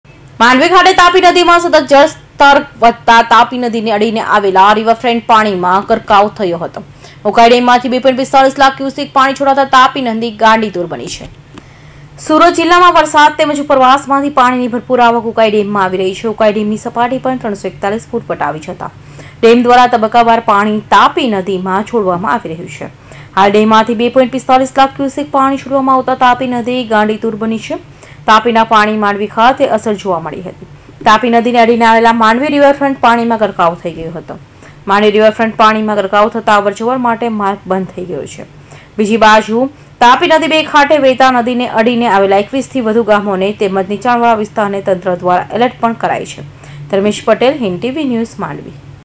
માંડવી તાપી નદી ને અડી ને આવેલ રિવરફેન્ટ પાણી માં ગરકાવ થયો
ઉકાઈ ડેમ માંથી 2.45 લાખ ક્યુસેક પાણી છોડાતાં તાપી નદી ગાંડીતુર બની